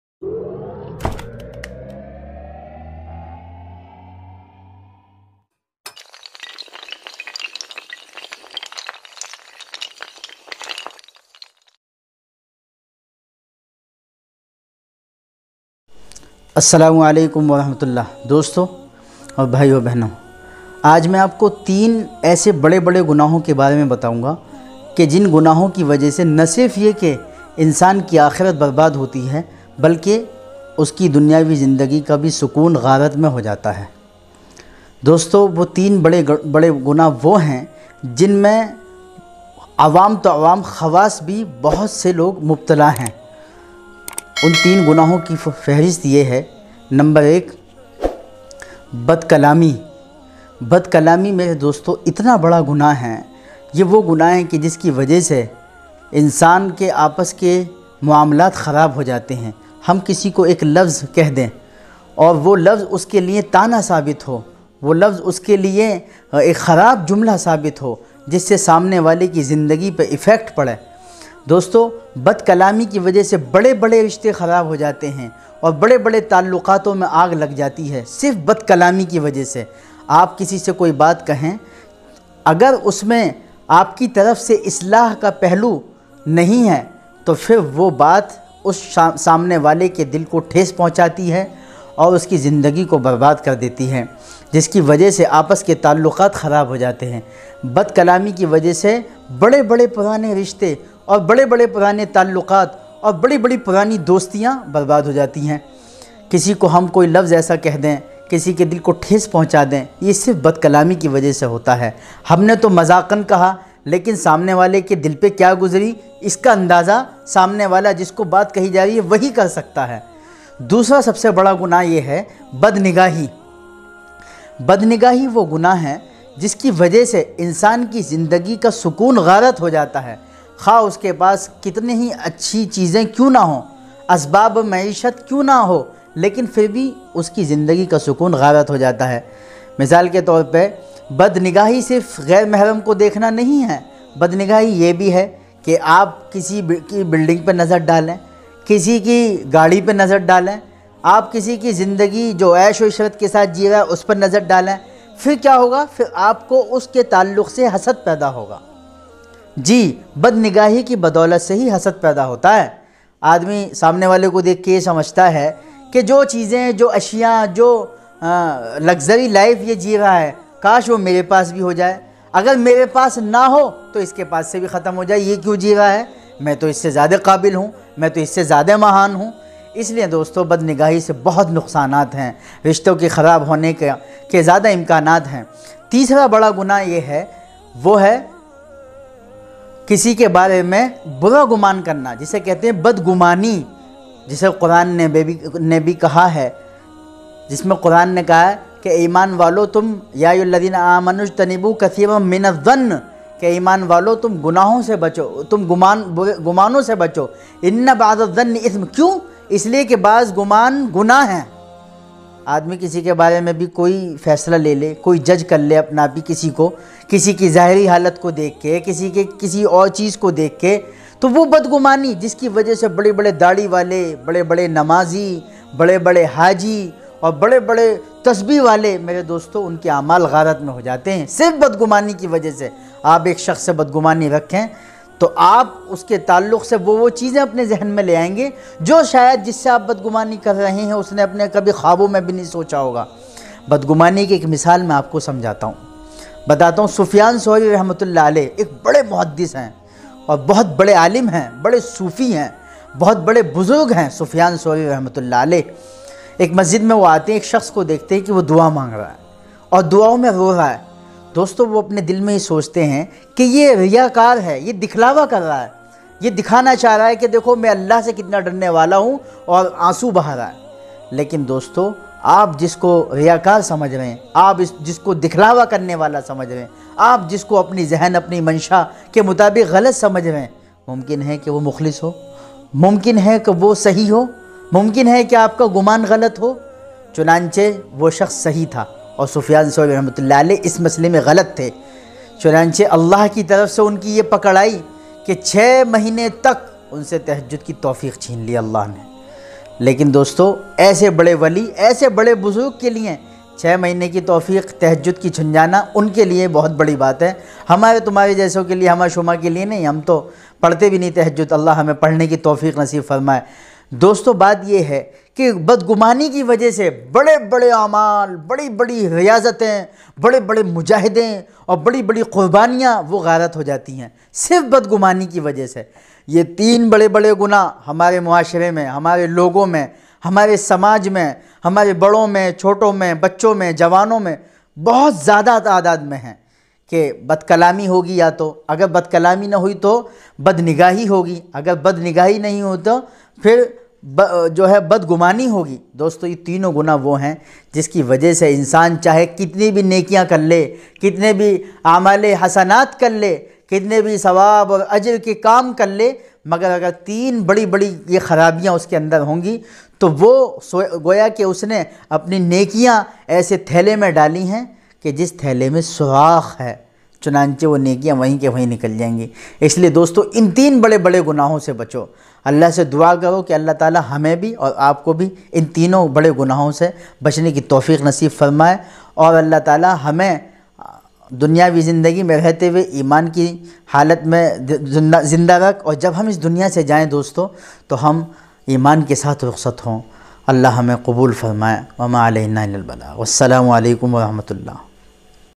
3 Bade bade gunah بڑے بڑے گناہ جو انسان کو برباد کر سکتے ہیں
Bade bade 3 gunah jo insan ko halak kar sakte hen

Unse bachna zaruri he

Please puri video ko dekhen

Or mujhe support bhi karen